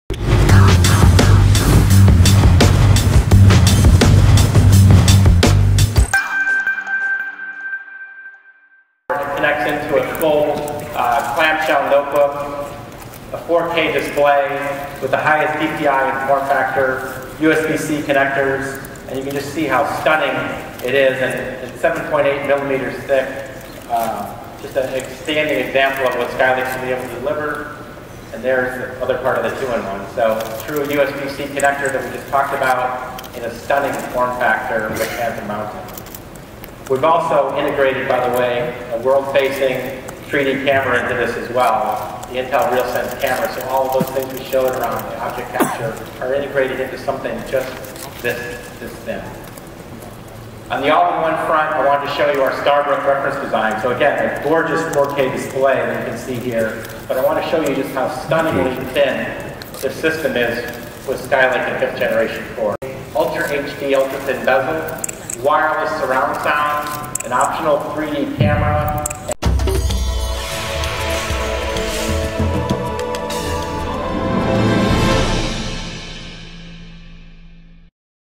Intel's crazy Skylake references devices at Computex
Intel briefly showed what's possible with Skylake at its Computex press conference, and the results are pretty astonishing.